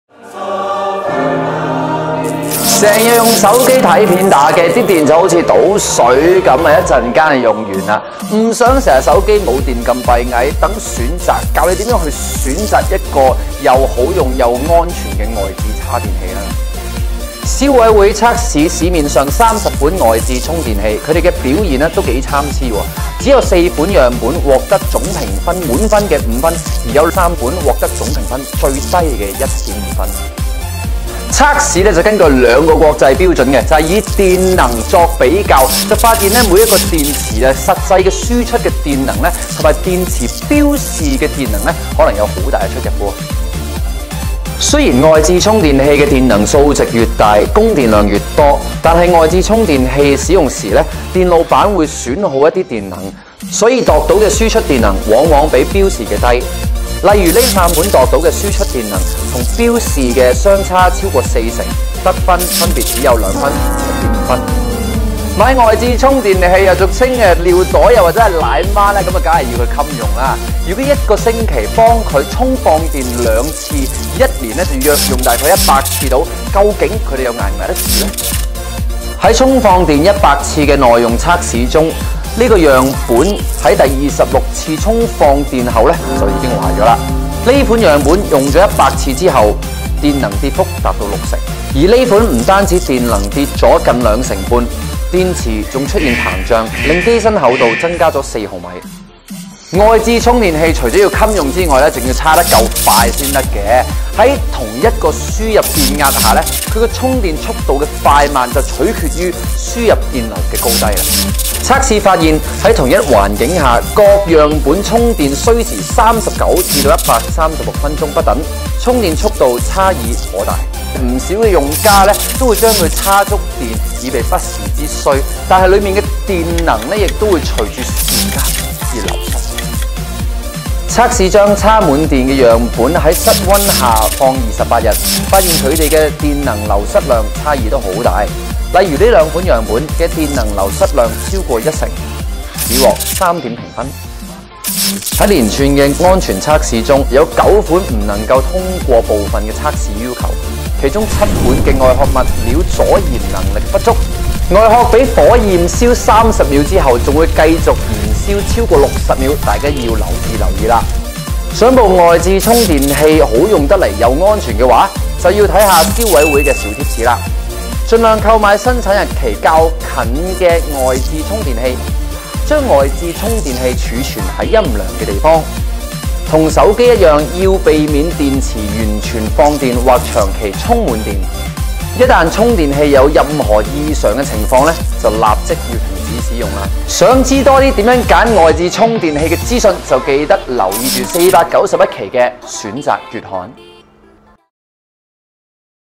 陳柏宇 x 外置充電器
成日用手機睇片同打機，電池好像「倒水」般一陣就用完。
不想手機沒電那麼煩惱，讓《選擇》教你選擇個安全又好用的外置充電器啦！

消委會測試市面上30款外置充電器，它們的表現都頗參差。
只有4款樣本獲得總評滿分的5分，而有3款只獲總評最低的1.5分。

測試根據兩個國際標準，以電能作比較，發現各個樣本實際輸出的電能(Wh)，和電池標示的電能，可以有很大出入。

雖然外置充電器的電能數值愈大，供電量愈多。
但外置充電器使用時，電路板會損耗一些電能，所以量得的輸出電能，往往比標示的低。

例如這3款量得的輸出電能，和標示相差超過4成，分別只得2分和1.5分。

買外置充電器，當然要夠耐用啦。如果一個星期幫它充放電2次，一年就用約100次，究竟它們能否捱得到呢？

在充放電100次的耐用測試中，這款樣本在第26次充放電後就壞了。

這款樣本使用100次後，電能跌幅達到6成

而這款不止電能跌了近兩成半(-24.5%)，電池還出現膨脹，令機身厚度增加了4毫米！

外置充電器除了要耐用之外，還要充電快才得，在同一輸入電壓下，它的充電速度快慢，取決於輸入電流的高低。

測試發現，在同一環境下，各樣本充電需時39-136分鐘不等，充電速度差異頗大

外置充電器不止要耐用，還要耐放
不少用家都會將它充滿電，以備不時之需，但裡面的電能會隨時間而流失。

測試將充滿電的樣本在室溫下放28日，發現它們的電能流失量差異很大。
例如這兩款樣本的電能流失量超過1成，只獲3點評分。

在連串的安全測試中，有9款未能夠通過部分測試要求。
其中這7款的外殼物料阻燃能力不足，外殼被火燄燃點30秒後，還會繼續燃燒超過60秒，大家要留意啦！

想部外置充電器好用又安全？睇下消委會嘅小貼士！
• 盡量購買生產日期較近的外置充電器
• 將外置充電器儲存喺陰涼於地方
• 和手機一樣，要避免電池完全放電或長期充滿電
• 一旦充電器有任何異常情況，應立即停止使用。

想知更多選擇外置充電器的資訊？記得留意491期《選擇》月刊！